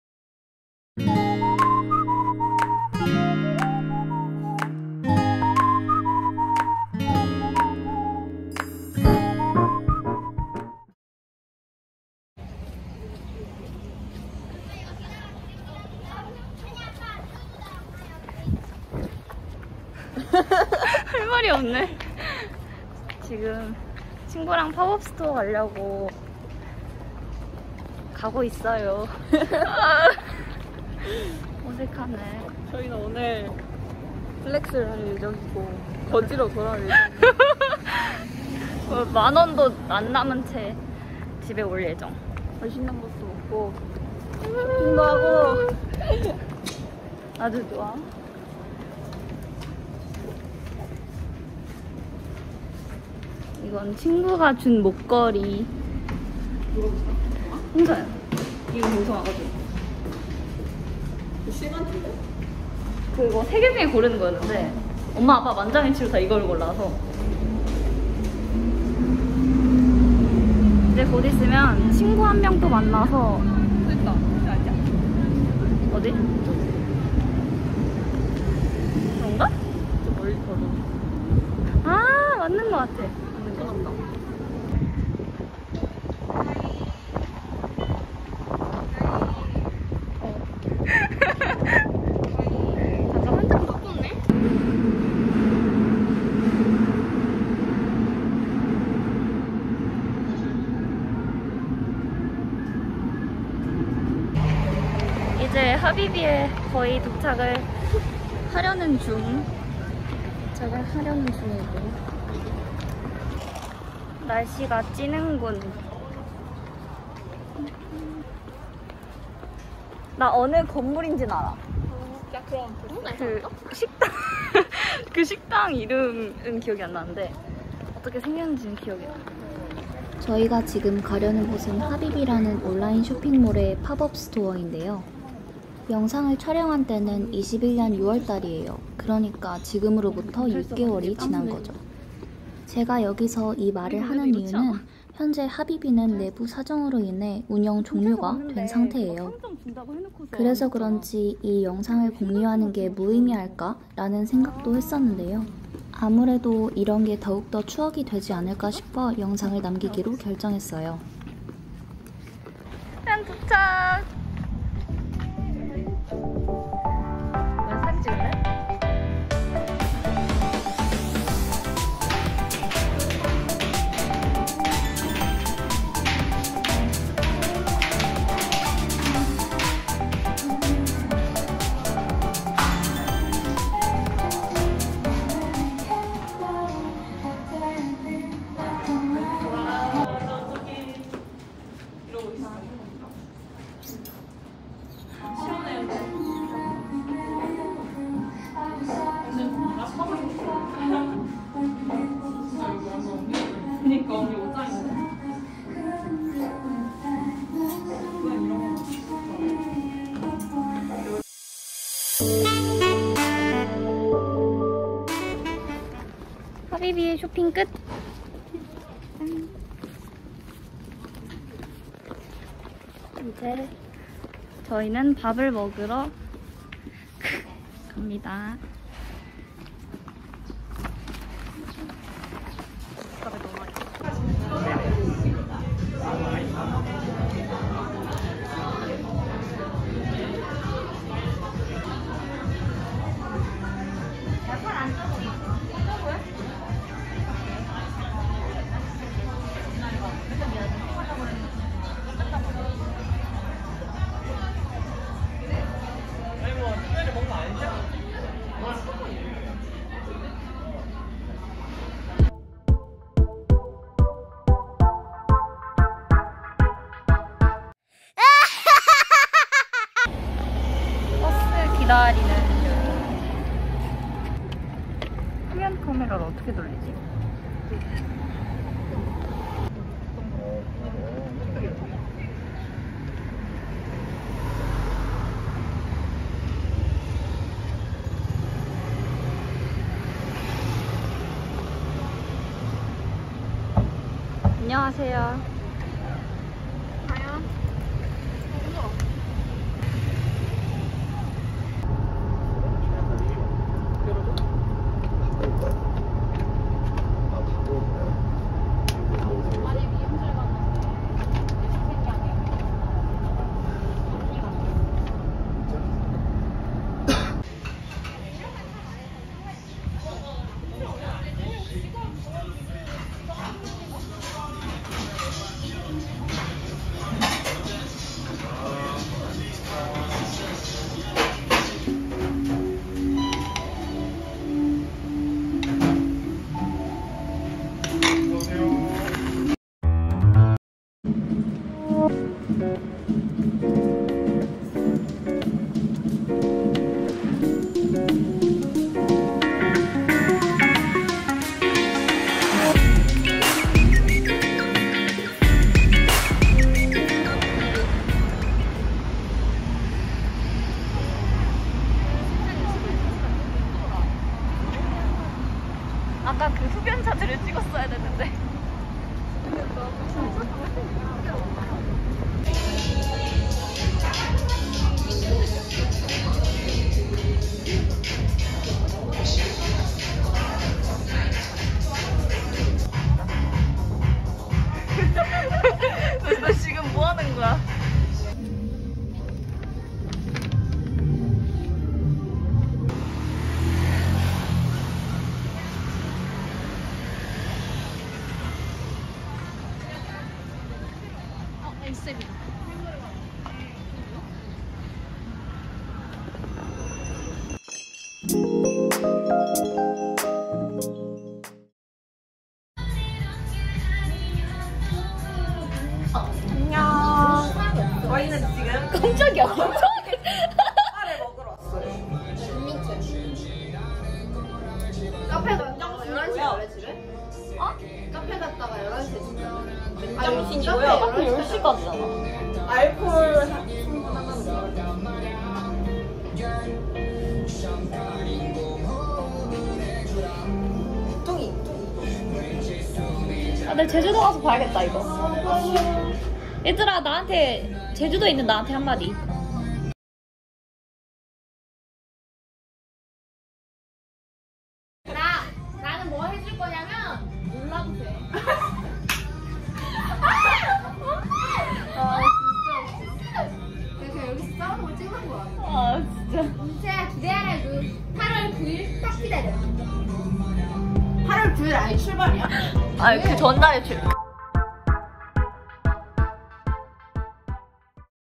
🛍 VLOG 제주로 떠나기 전 친구들과 좋은날 | HABIBI/하비비 팝업스토어 | 쇼핑, 디저트, 국물 닭발, 수다 ❤️
나의 소중한 하나뿐인 친구들,